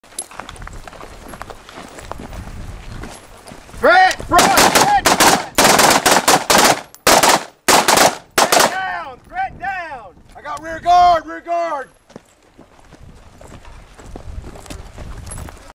Hoffners Rifle Team Assault and Rear Guard Post
Though the entire team may engage the known threat, someone must move to rear guard ASAP! Keep the 540 covered, Bad Guy Rule #1...Where there is one there are two.  Train Hard & Keep your head on a spindle, Hoff Hoffners Training Academy, Houston Texas